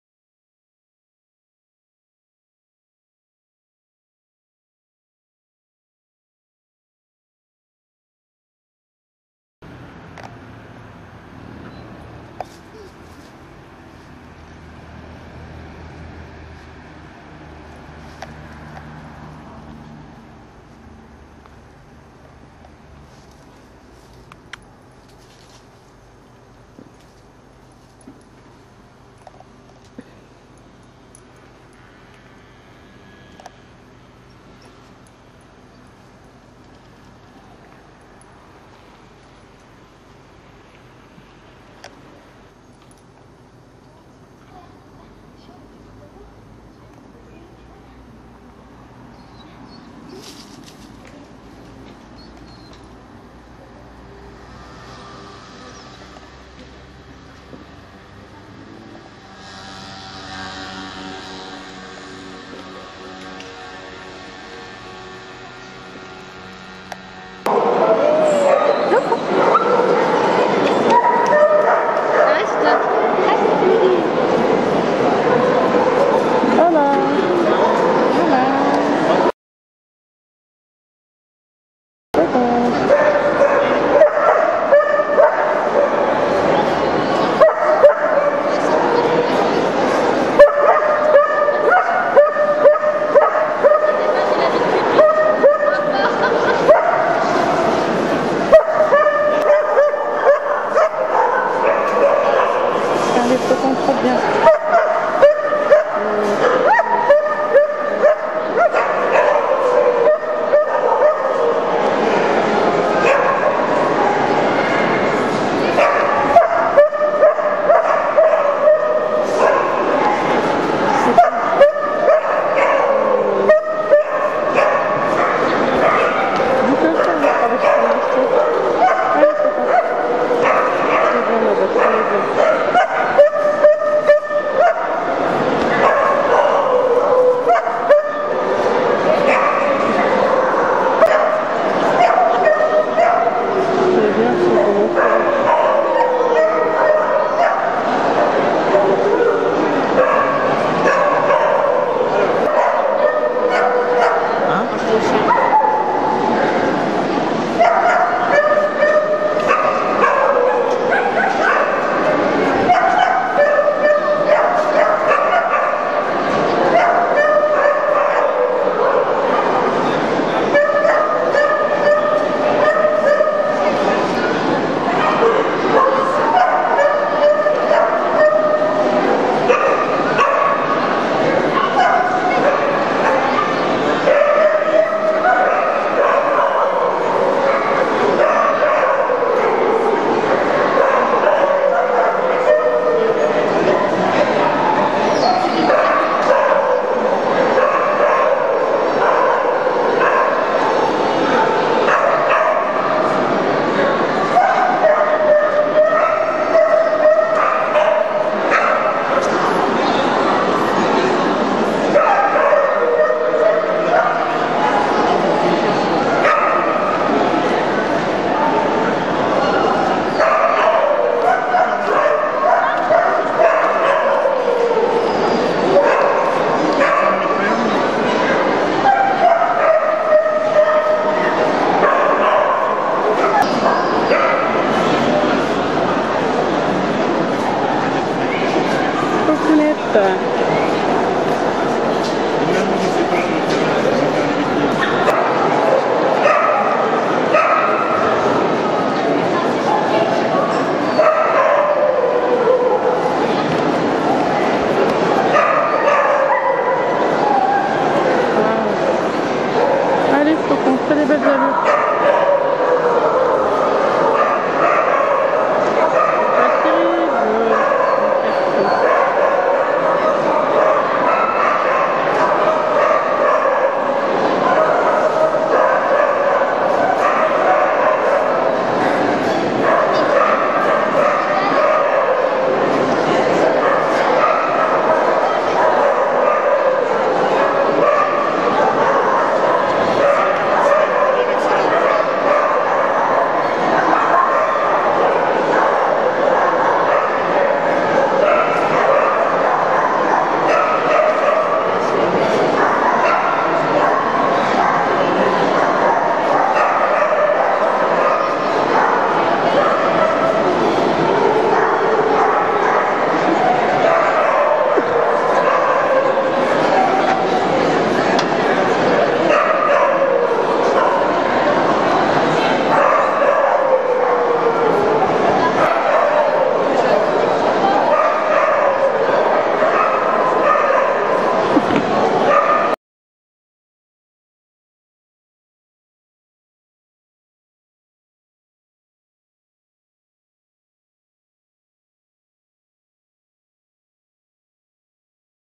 Exposition Canine - Flocon des Ecrins de Sirius
Exposition Nationale à La Roche-sur-Foron, le 28 août 2011 (Exposition en Classe Intermédiaire et Confirmation)